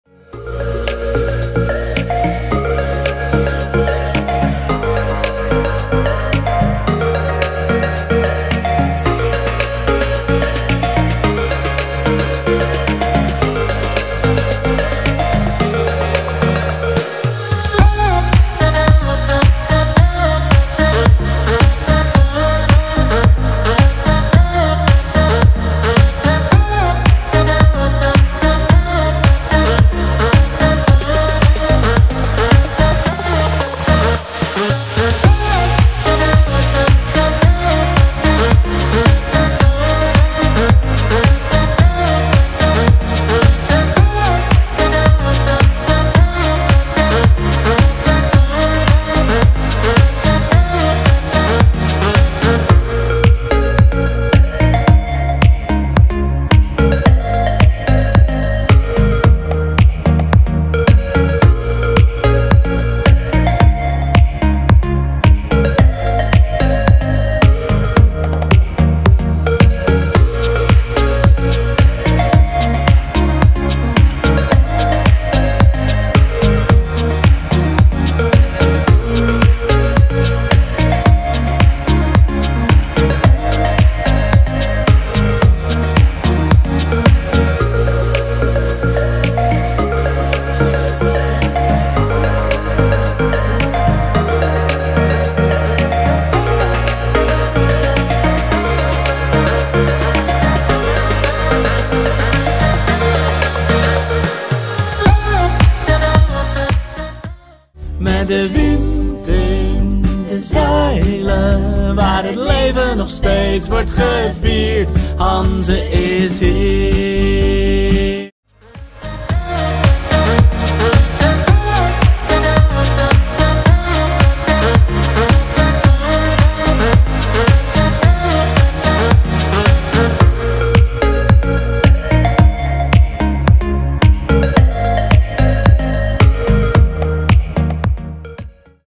Hanzejaar2023 - Resultaten
2023 was het Hanzejaar: 9 maanden lang is met 9 steden het Hanze-gedachtengoed zichtbaar en beleefbaar gemaakt. Als aanjager en impuls voor een duurzaam economisch herstel, brede welvaart en voor groei van trots bij bewoners.

De inzet: een toeristisch-recreatief en cultureel interessant jaar dat bezoekers en bewoners trekt en vooral de basis legt voor een maatschappelijk relevante samenwerking in de toekomst. Inmiddels is het jaar afgesloten en blikken we terug op dit bijzondere themajaar.